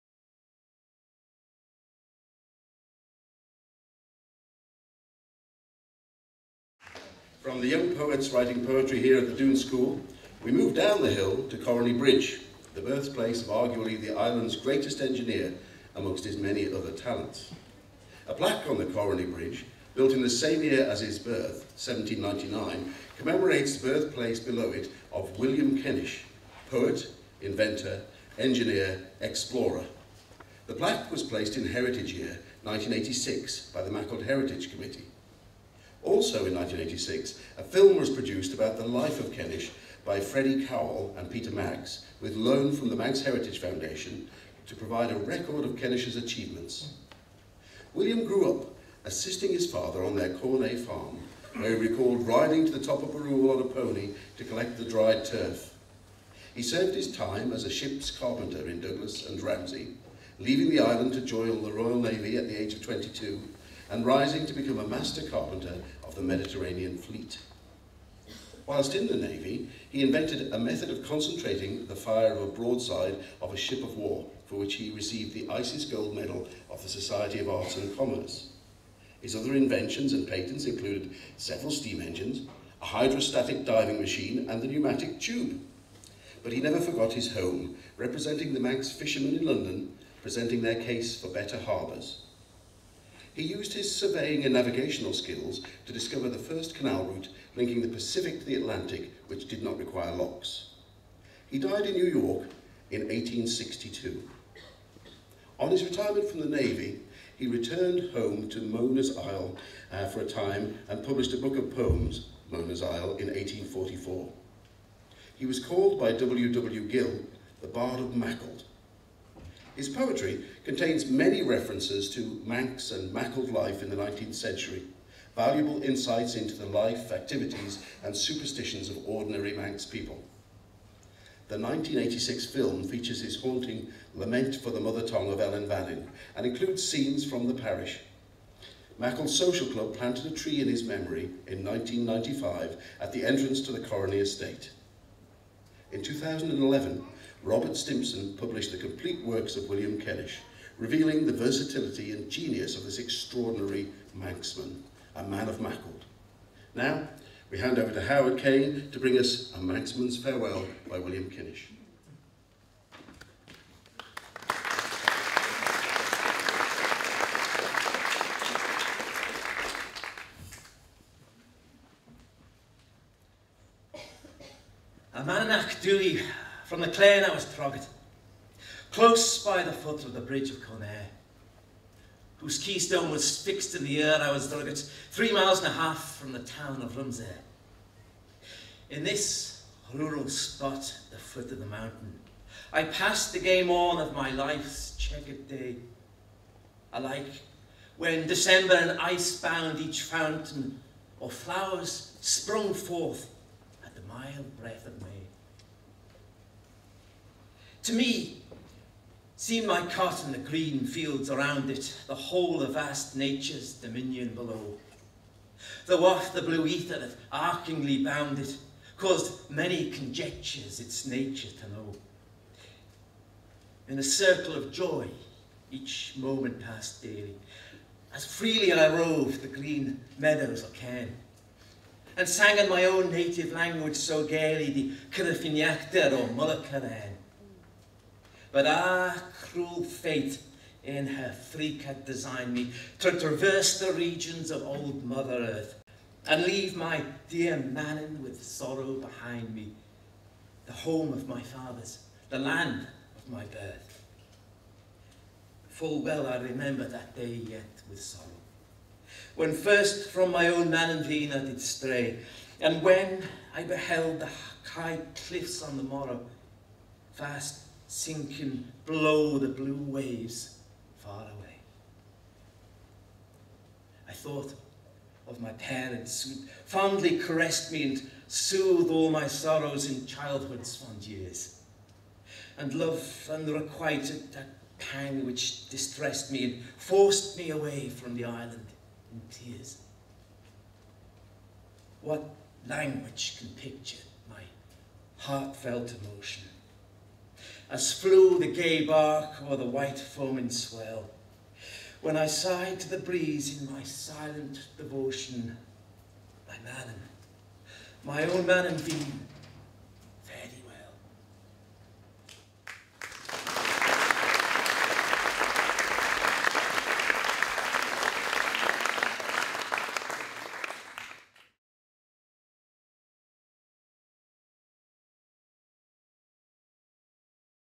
"A Manxman’s Farewell" by William Kennish read by Howard Caine [The Maughold Show, Pt. 10]
Maughold parish’s inventor poet is remembered with Howard Caine reading ‘A Manxman’s Farewell’.

The Maughold Show: A Celebration of the People, Poetry, Music and Mysticism of Maughold Parish
Organised by Maughold Social Club and the Dhoon School for Island of Culture 2014

We acknowledge the copyright interest of Manx National Heritage in some of the images used in this programme.

Filmed by Culture Vannin Friday 3rd October 2014